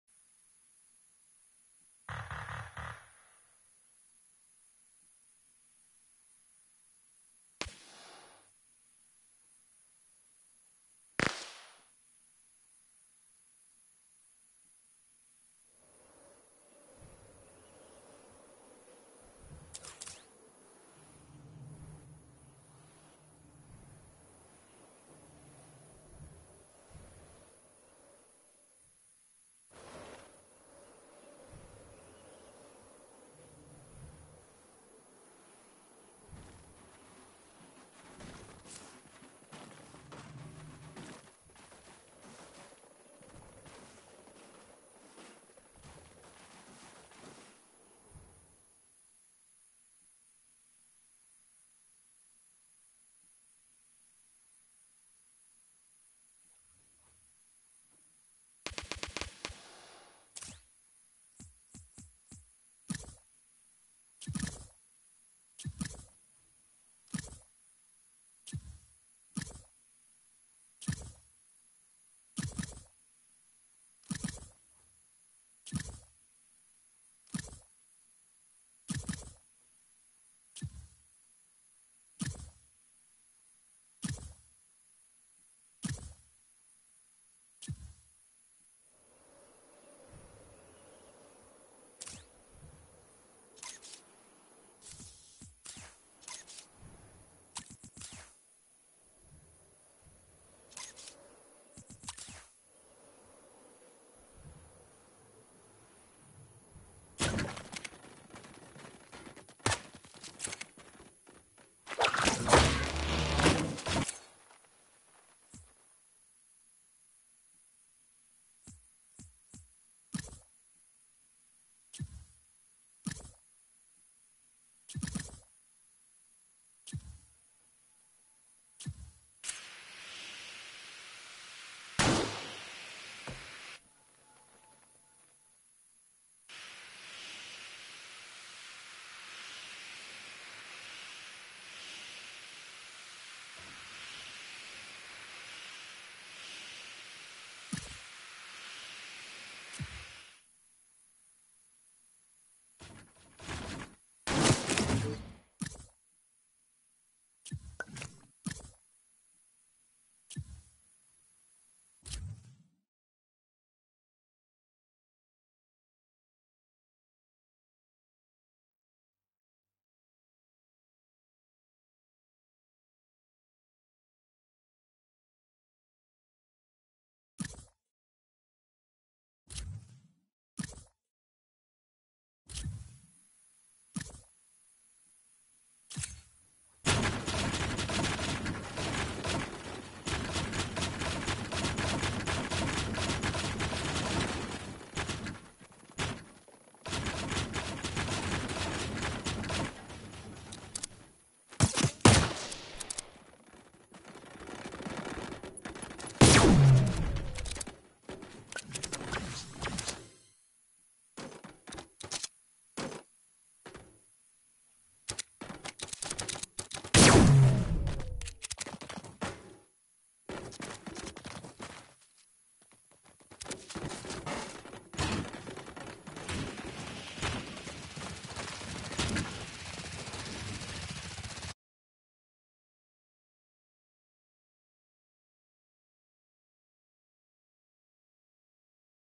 MarMar12398's Live PS4 Broadcast
Building max height stairway to heaven in playground!!    BCC Trolling!!